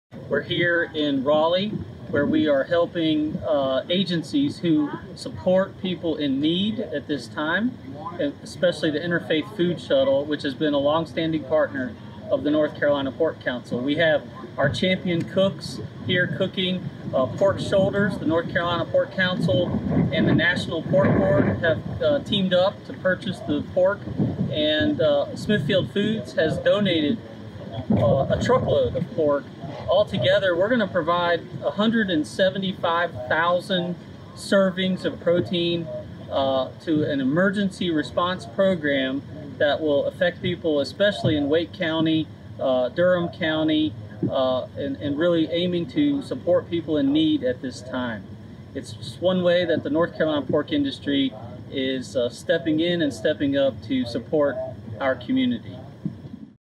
NC Pork and Whole Hog Barbecue Series Cooks team up for hunger relief
On April 17, the NC Pork Council and the Pork Checkoff provided 130 pork shoulders for nine Whole Hog Barbecue cooks to cook up for hunger relief efforts at Inter-Faith Food Shuttle in Raleigh. A truckload of pork was delivered the same day from Smithfield Foods' Helping Hungry Homes initiative.